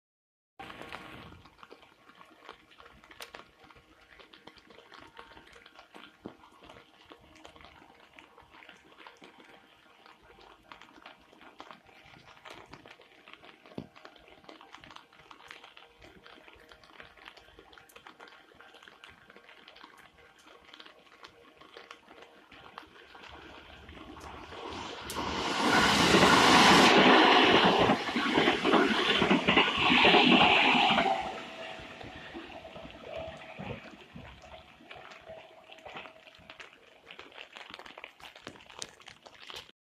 雨の近鉄京都線山田川駅 1編成しかいない近鉄8600系6両編成
急行橿原神宮前行き
近鉄8600系X69編成